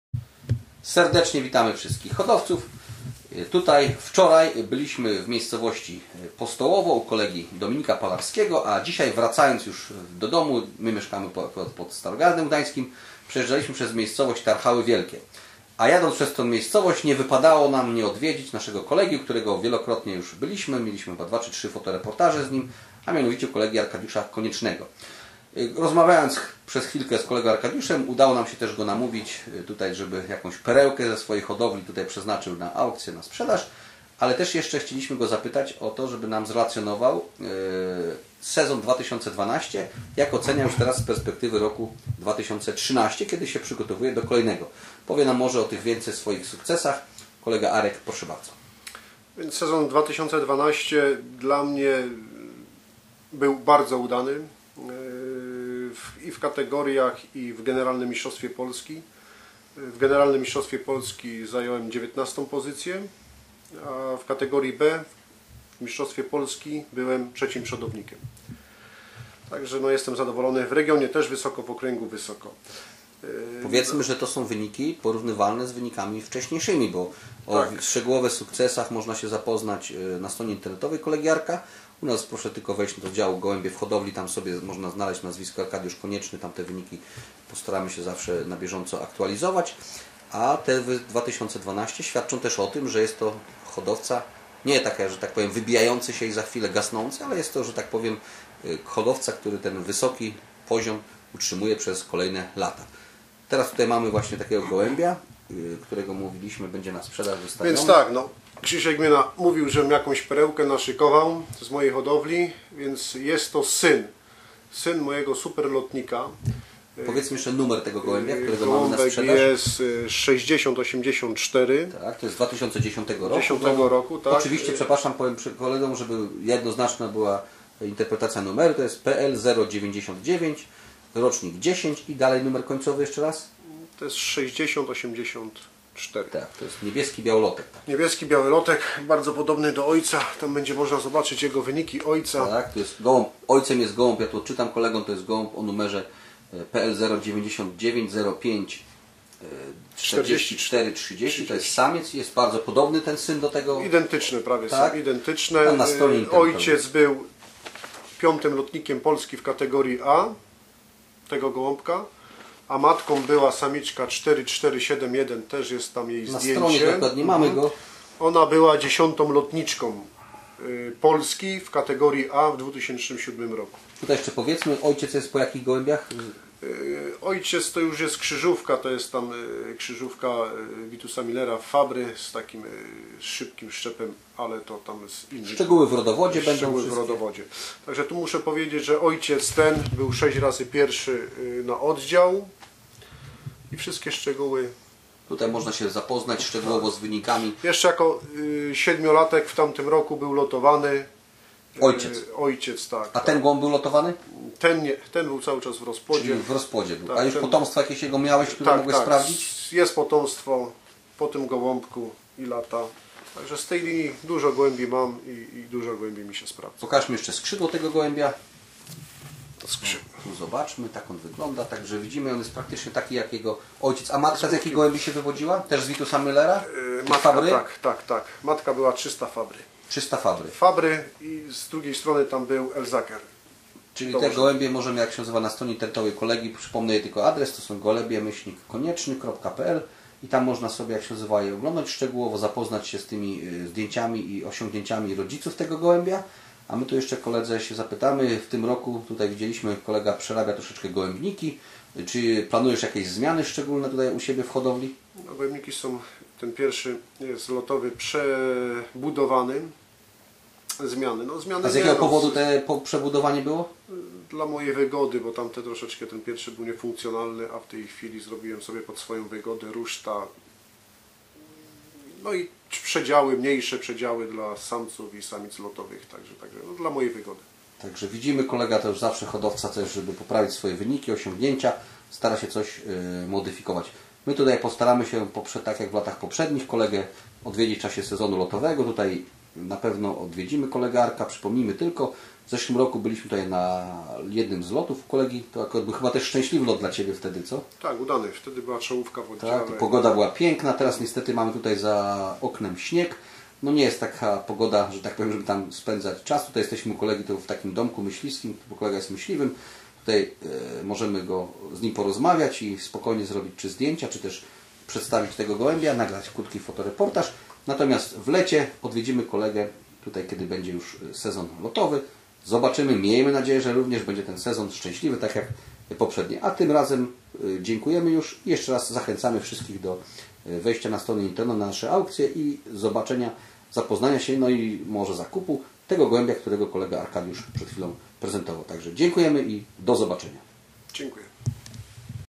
Arkadiusz Konieczny - oddział PZHGP 099 - Odolanów - luty 2013
Arkadiusz Konieczny - oddział PZHGP 099 - Odolanów - luty 2013 - prezentacja gołębia na sprzedaż po superlotnikach.